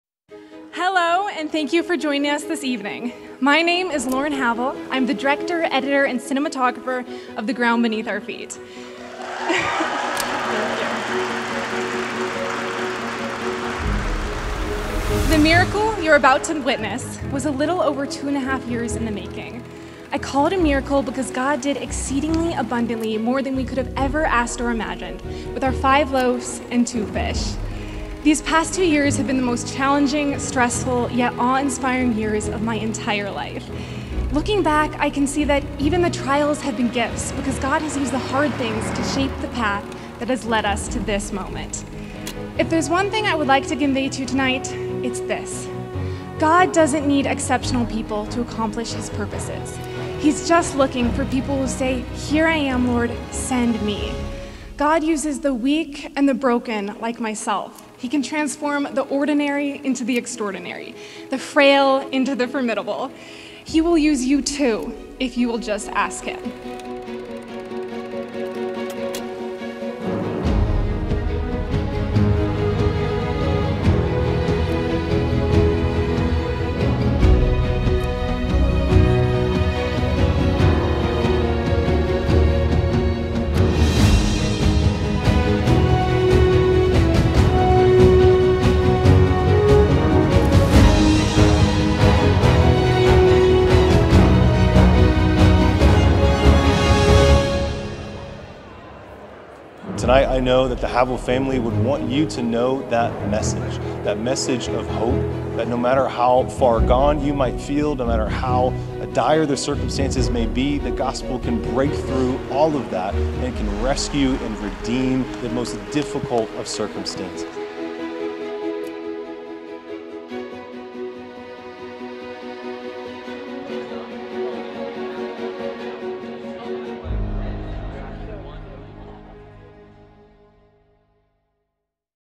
The Ground Beneath Our Feet | WORLD PREMIERE
The world premiere of Lauren Grace Studios' brand new film, 'The Ground Beneath Our Feet' took place on June 11th, 2024 at the Morrison Center of Performing Arts. We hope you enjoy this recap of a truly unforgettable night!

A huge thank you to @SimonDunnProductions for the incredible footage of the event!